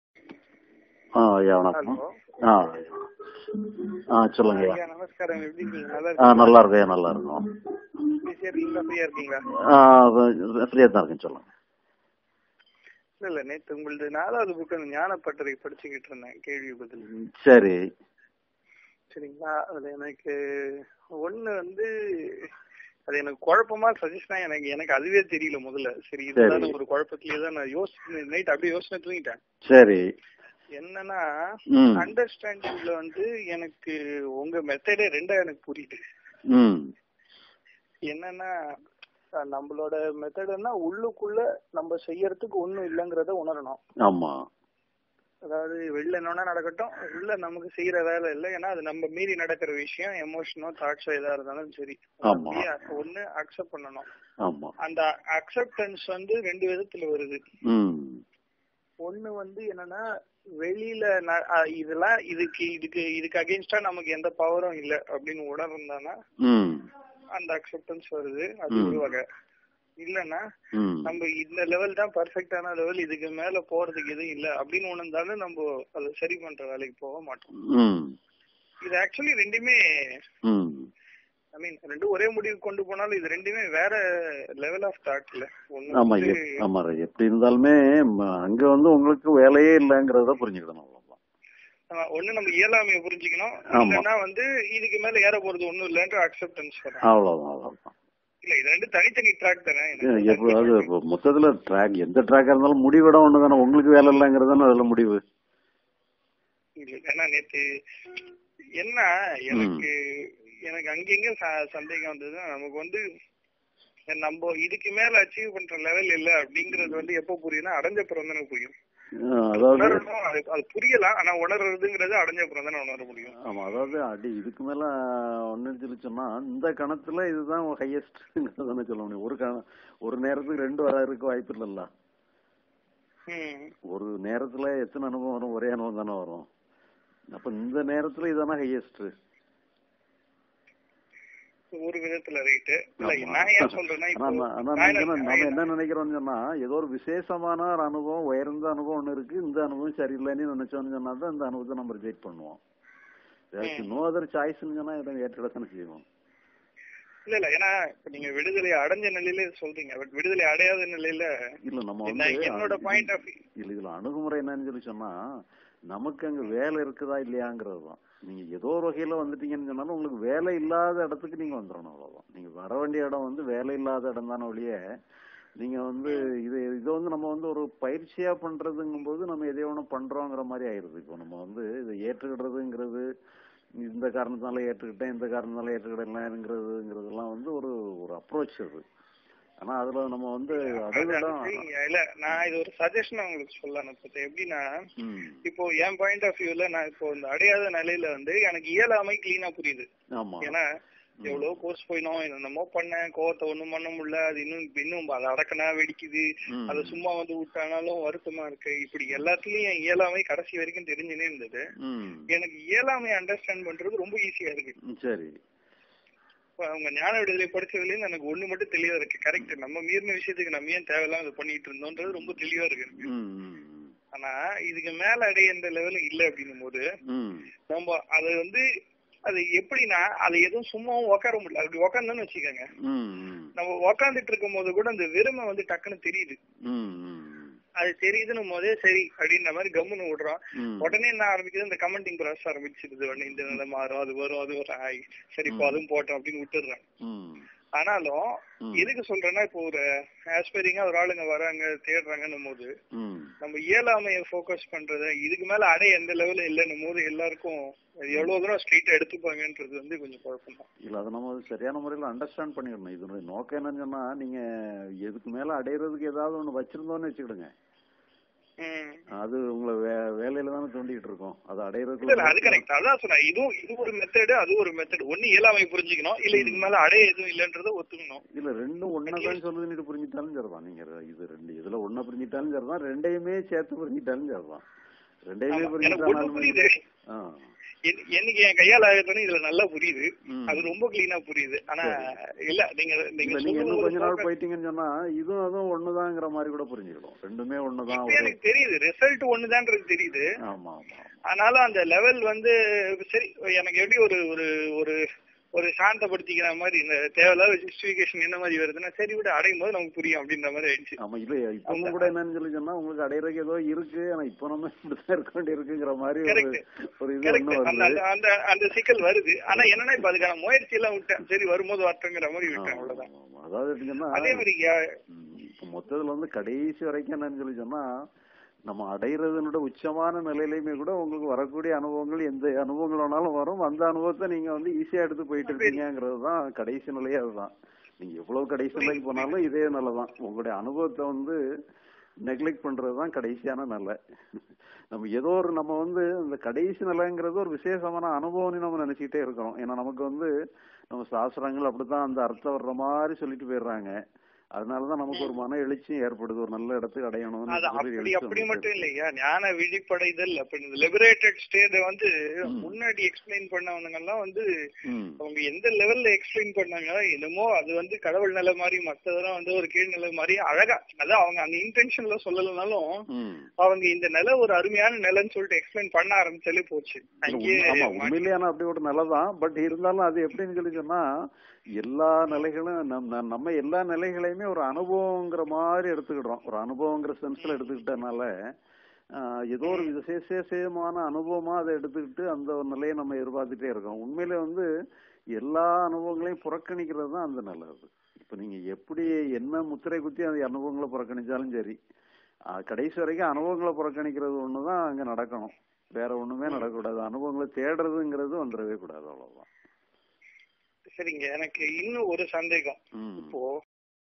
011218 இயலாமை Incapable Q&A Sri Bagavath Tamil
Sri பகவத் ஐயா Introduction Whatsapp குழுவில் இணைய ஞானப்பட்டறையிலிருந்து வந்த குழப்பமான நிலை. , இயலாமை அல்லது இதற்கு மேல் எதுவும் இல்லை என்ற நிலை வேண்டுமா ?    அணுகுமுறை அங்கே வேலை உண்டா இல்லயா என்பதுதான் கேள்வி. அனுபவங்களை easy யாக எடுத்து போக வேண்டியுள்ளது.